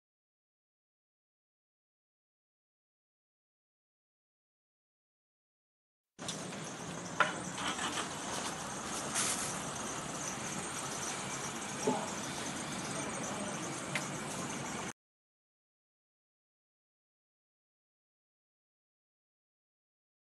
Airlock Sieve
vibrating screen manufacturer from China
Xinxiang Techeng machinery and Equipment Co., Ltd.
Linkman：Keiven Hao
Vibro seive  is a vibratory screening device that vibrates about its center of mass. Vibration is accomplished by eccentric weights on the upper and lower ends of the motion-generator shaft. Rotation of the top weight creates vibration in the horizontal plane, which causes material to move across the screen cloth to the periphery. The lower weight acts to tilt the machine, causing vibration in the vertical and tangential planes. The angle of lead given the lower weight with relation to the upper weight provides variable control of the spiral screening pattern. Speed and spiral pattern of material travel over the screen cloth can be set by the operator for maximum throughput and screening efficiency of any screenable product .